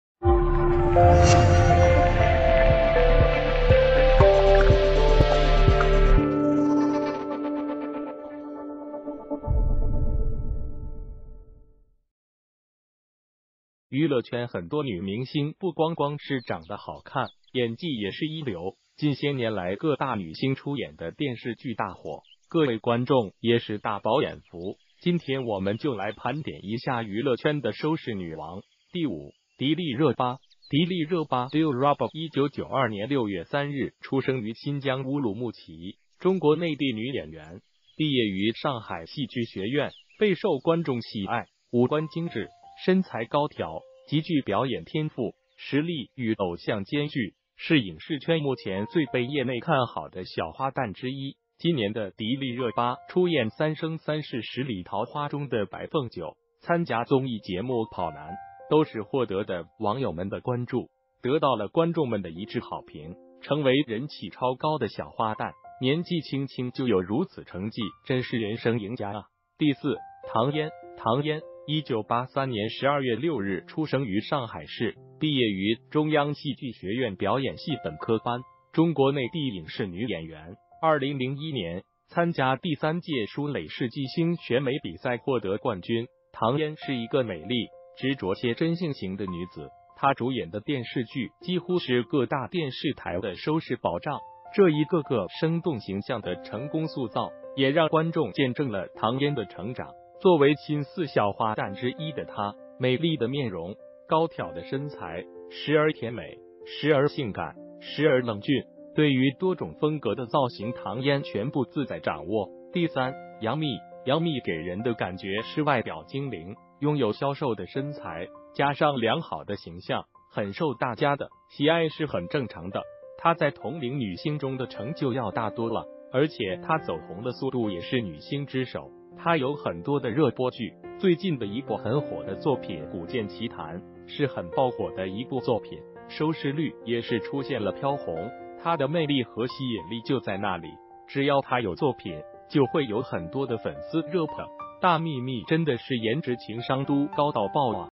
娱乐圈收视女王，热巴第五，范冰冰第二，第一是她！
谢谢！娱乐圈很多女明星不光光是长的好看，演技也是一流，近些年来各大女星出演的电视剧大火，各位观众也是大饱眼福。今天我们就来盘点一下娱乐圈的收视女王。第五，迪丽热巴。迪丽热巴(Dilraba)，1992年6月3日出生于新疆乌鲁木齐，中国内地女演员，毕业于上海戏剧学院。备受观众喜爱，五官精致，身材高挑，极具表演天赋，实力与偶像兼具，是影视圈目前最被业内看好的小花旦之一。今年的迪丽热巴出演《三生三世十里桃花》中的白凤九，参加综艺节目《跑男》，都是获得的网友们的关注，得到了观众们的一致好评，成为人气超高的小花旦!年纪轻轻就有如此成绩真是人生赢家啊!第四，唐嫣。唐嫣，1983年12月6日出生于上海市，毕业于中央戏剧学院表演系本科班，中国内地影视女演员。2001年，参加第三届舒蕾世纪星选美比赛获得冠军。唐嫣是一个美丽、执着且真性情的女子。她主演的电视剧几乎是各大电视台的收视保障，这一个个生动形象的成功塑造，也让观众见证了唐嫣的成长。作为“新四小花旦”之一的她，美丽的面容、高挑的身材，时而甜美、时而性感、时而冷峻，对于多种风格的造型唐嫣全部自在掌握。第三，杨幂。杨幂给人的感觉是外表精灵、拥有消瘦的身材，加上良好的形象，很受大家的喜爱是很正常的。她在同龄女星中的成就要大多了，而且她走红的速度也是女星之首。她有很多的热播剧，最近的一部很火的作品《古剑奇谭》是很爆火的一部作品，收视率也是出现了飘红。她的